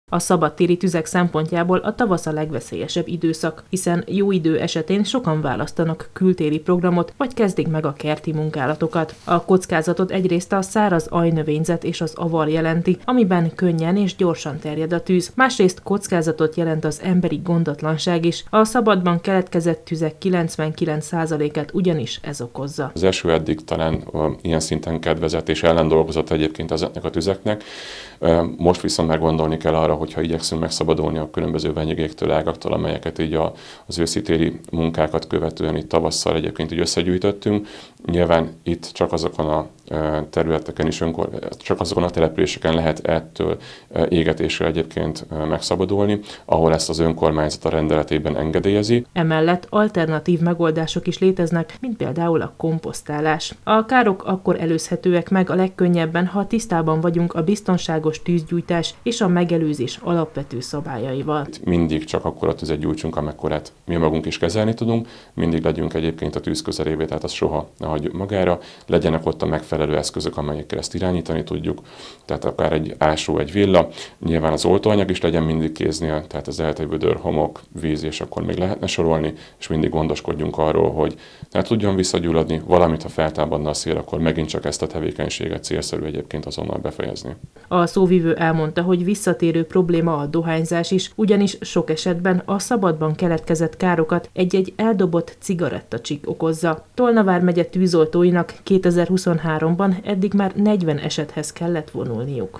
Egyre több a szabadtéri tűz - Fontos a megelőzés és az alapvető szabályok ismerete
A tavasz kezdetével sokan választanak kültéri programot, vagy kezdik meg a kerti munkálatokat. Tolna vármegye lánglovagjai 2023-ban eddig már negyven szabadtéri tűzesethez vonultak. Szakemberek szerint, a károk akkor előzhetőek meg a legkönnyebben, ha tisztában vagyunk a biztonságos tűzgyújtás és a megelőzés alapvető szabályaival.